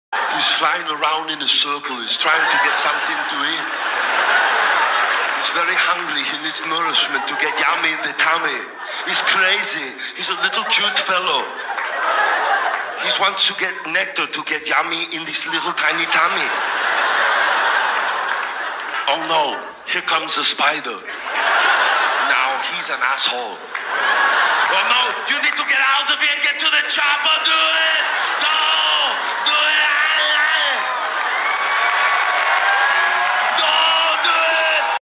Close up of a spider "Caught in it's own web"
"Caught in It's own web" is a short created to see close up how a spider moves. It felt threatened so it was on the move and released some web. The web got caught up in the spiders back legs.